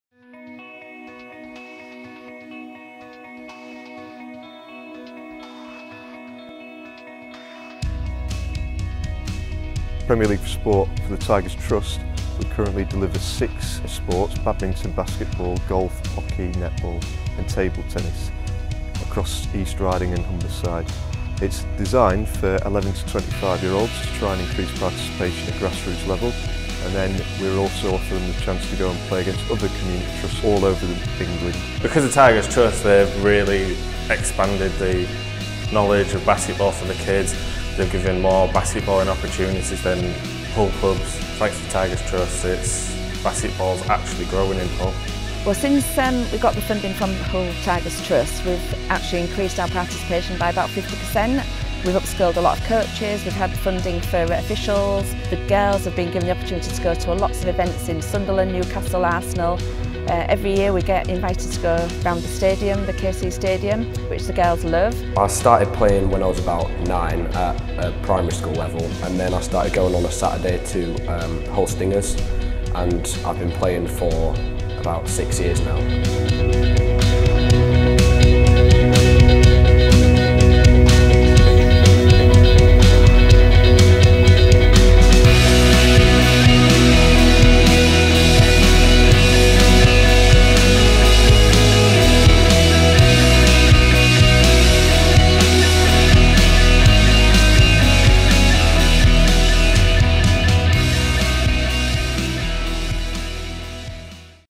Tigers Trust: Premier League For Sport
Sports session for young people aged 11-25 years, held at various venues across Hull and East Riding.

Our Premier League 4 Sport programme encourages you to get involved in sport. Take part for fun, to make friends, keep active or to progress in a particular sport.

We have a number of multi-sport clubs and many sport specific clubs, including: badminton, basketball, golf, hockey, netball, table tennis and tennis. Clubs run for 30 weeks per year, usually term time only, however, holiday activities will also be provided. Local, regional and national competitions will also be arranged.